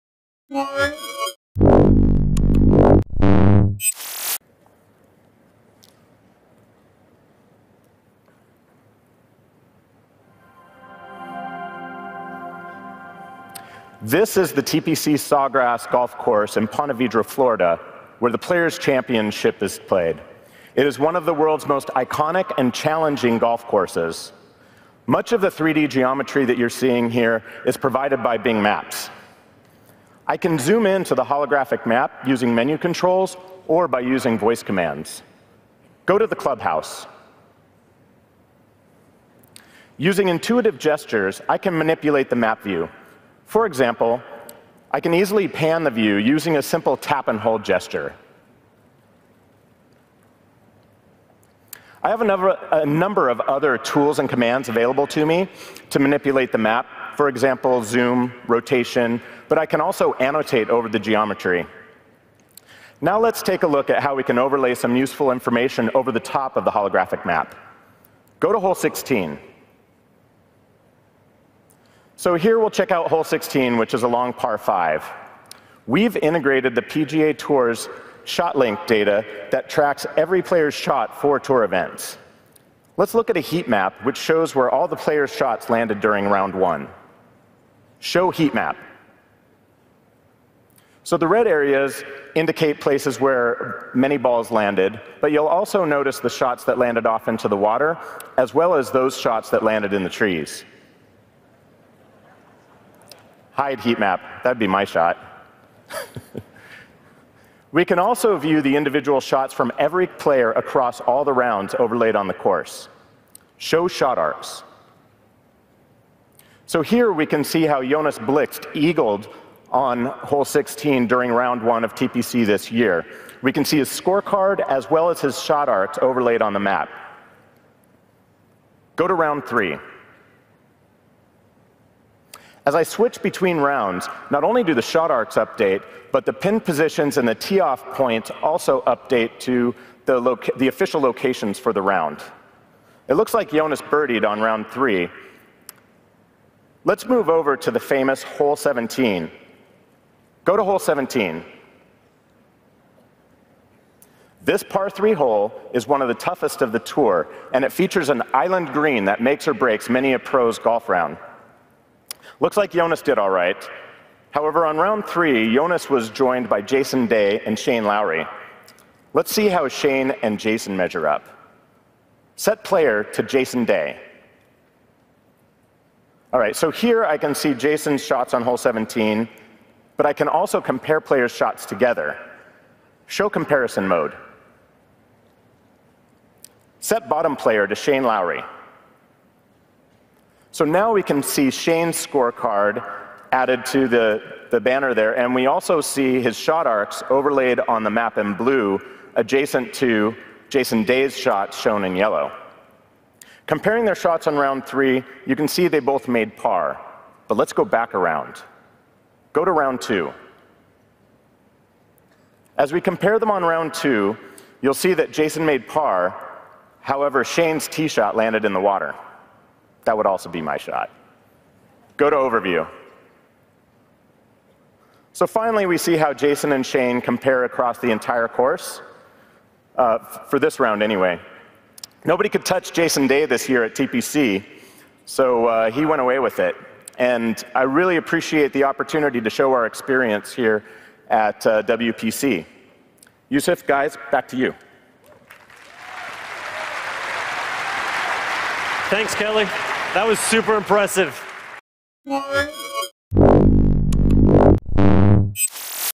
Amazing HoloLens PGA Demo by Taqtile ay WPC 2016
HoloLens PGA Demo - Check out this amazing HoloLens demo, Taqtile showed on day 2 at the Microsoft WPC 2016 in Toronto.